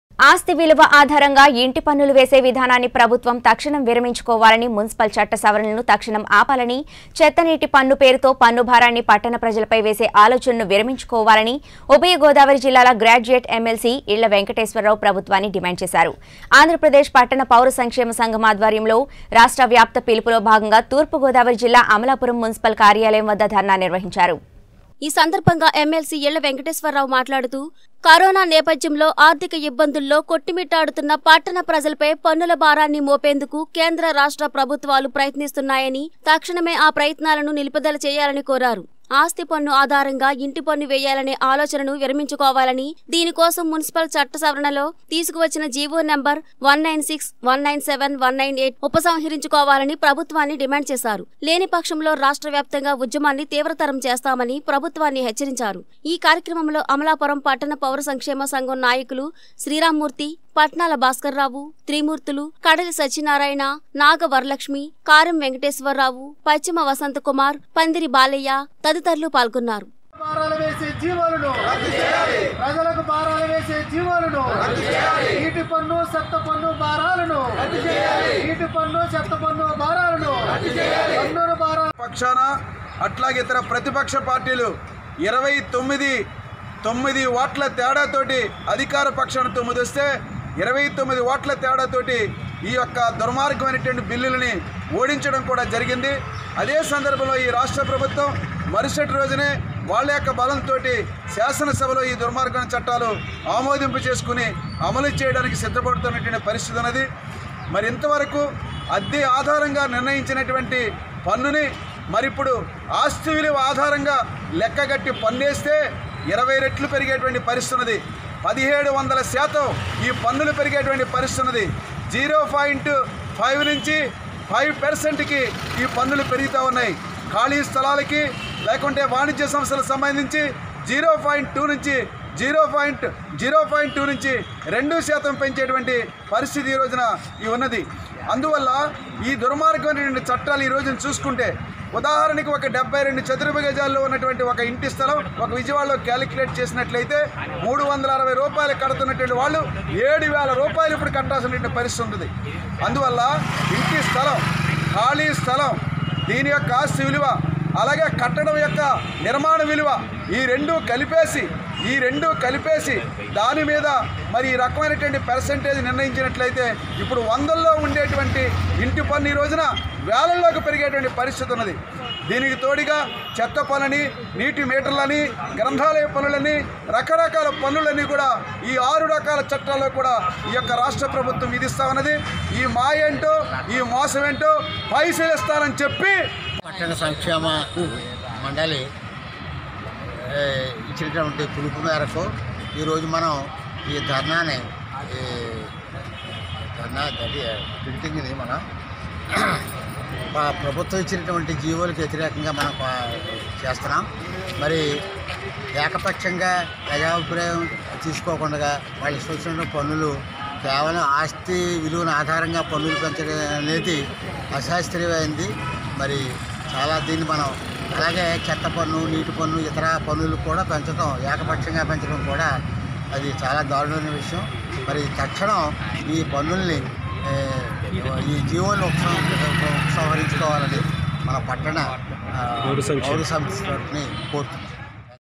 ఆస్తి విలువ ఆధారంగా ఇంటి పన్నులు వేసే విధానాన్ని ప్రభుత్వం తక్షణం విరమించుకోవాలి అమలాపురం ఎమ్మెల్సీ
ఆస్తి విలువ ఆధారంగా ఇంటి పన్నులు వేసే విధానాన్ని ప్రభుత్వం తక్షణం విరమించుకోవాలి అమలాపురం ఎమ్మెల్సీ ఇళ్లా వెంకటేశ్వరరావు తూర్పు గోదావరి జిల్లా | 19 Newstelugu